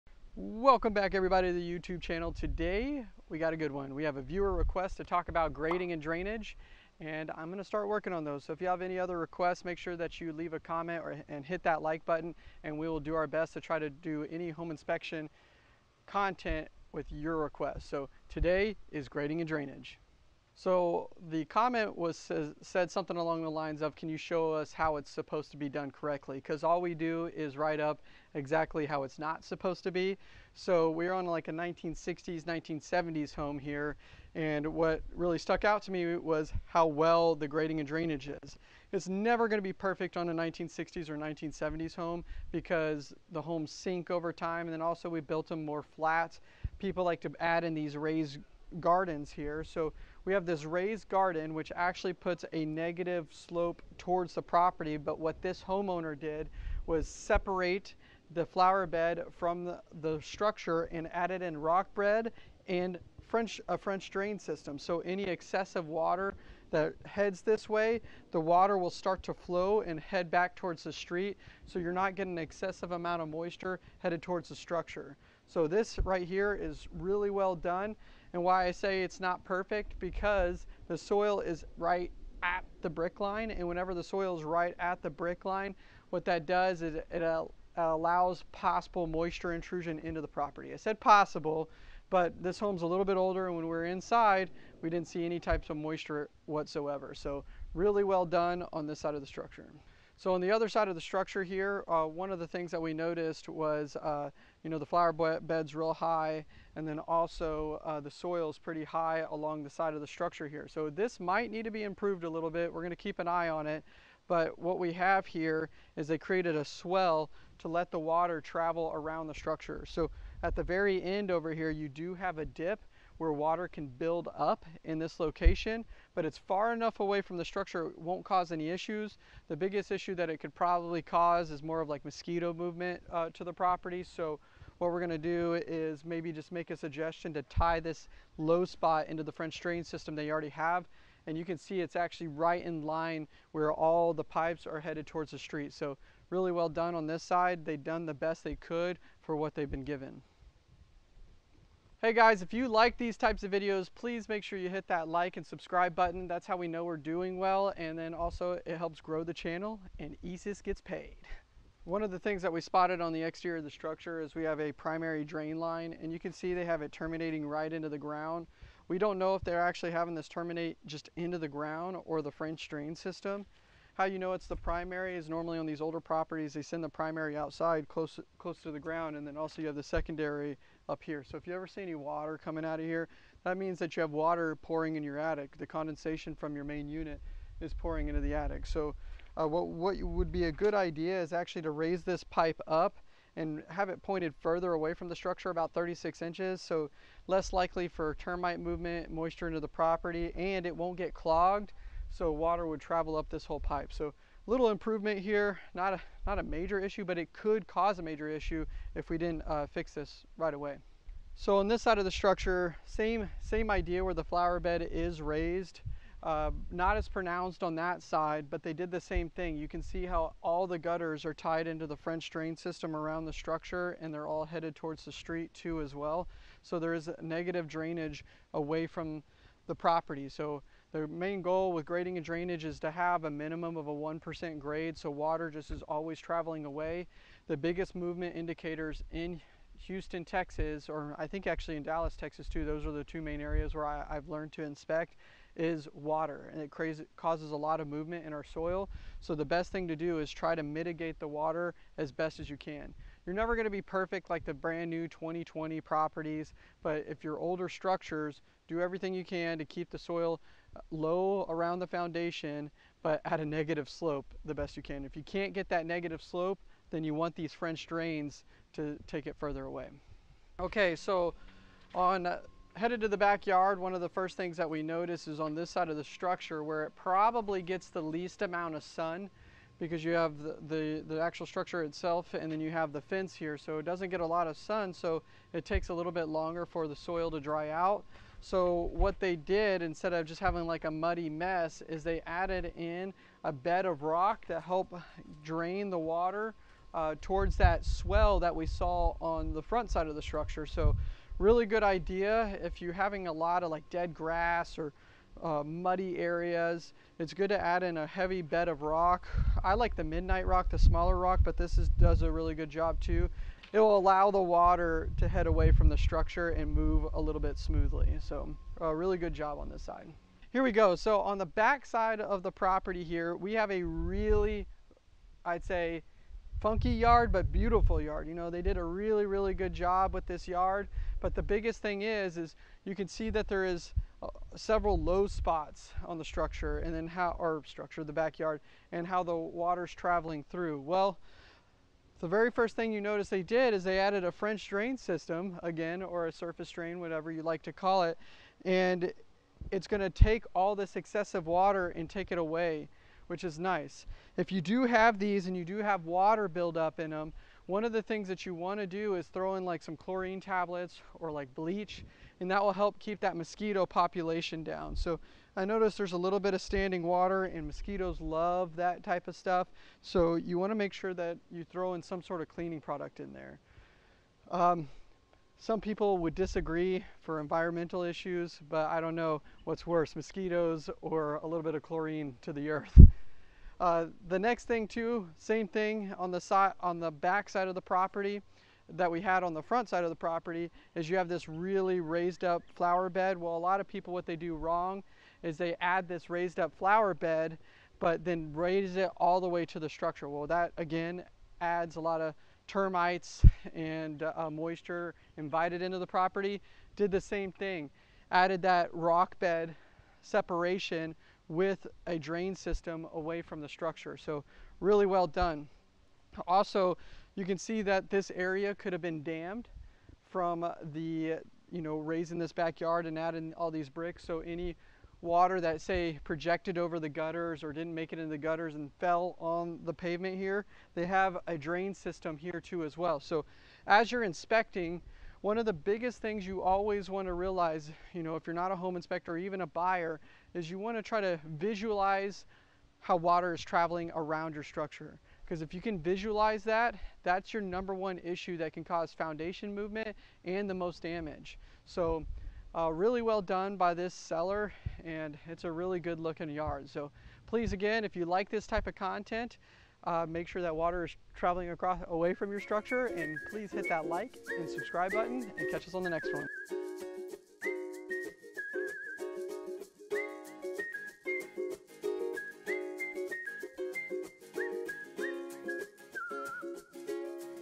Proper Grading and Drainage On An Old Structure
This week we're coming at you with a video all about proper grading and drainage. We got a comment on one of our last videos asking us to go over grading and drainage. Chris discusses proper grading and drainage on this older property. Let's go check it out!

A-ActionHouston.com
HoustonTexasHomeInspection.com

Next Youtube video out come and check it out.